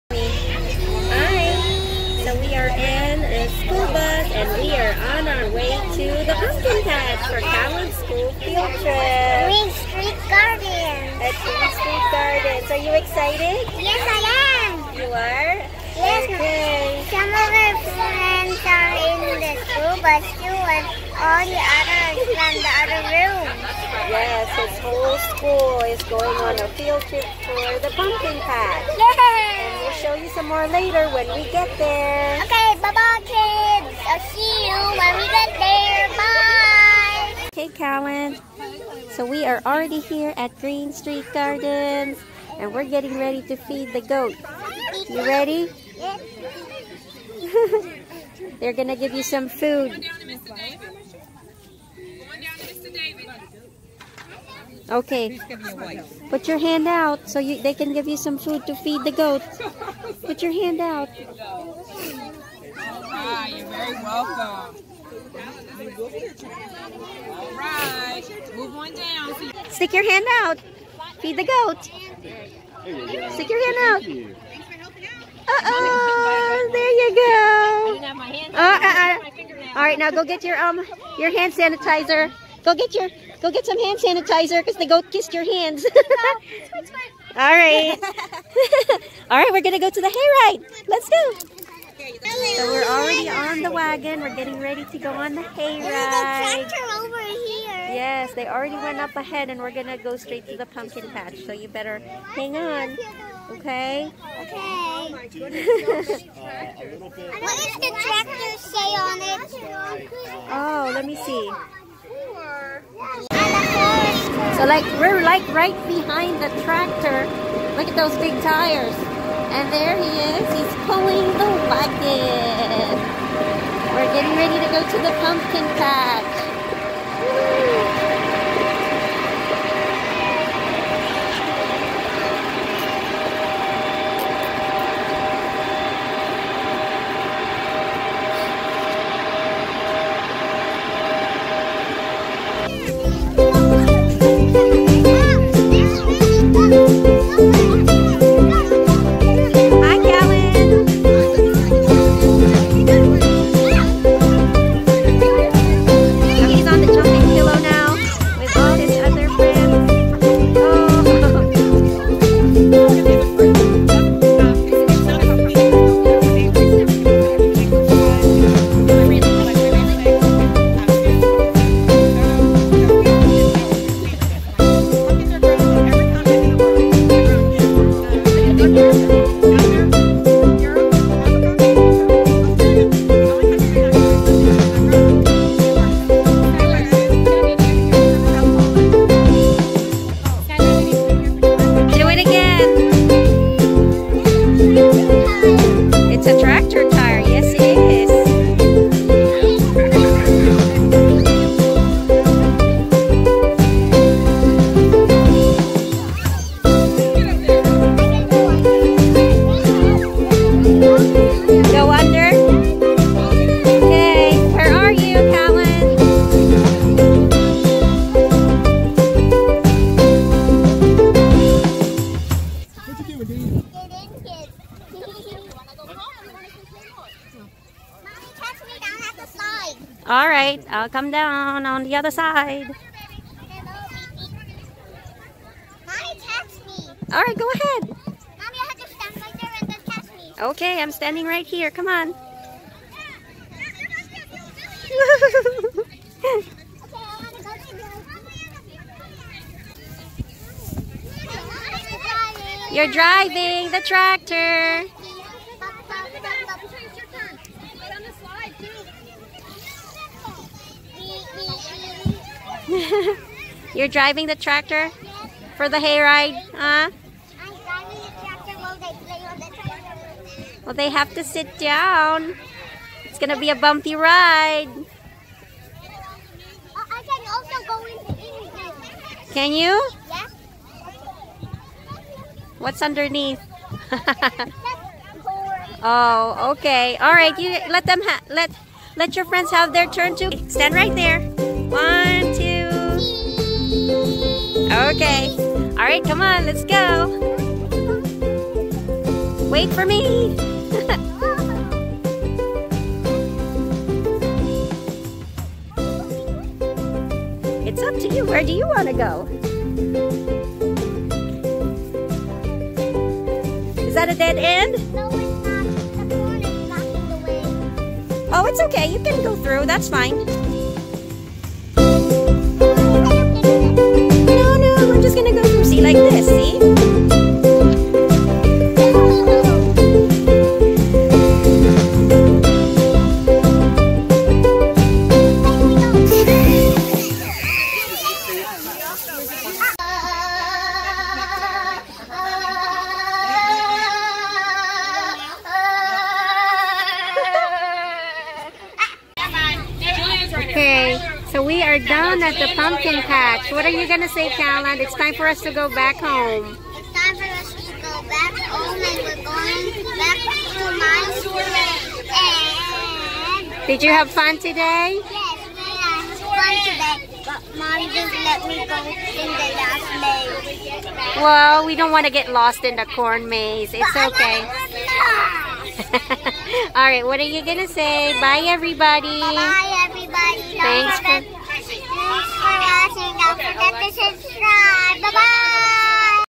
Pumpkin Patch Fun!
Cal's school field trip at the pumpkin patch.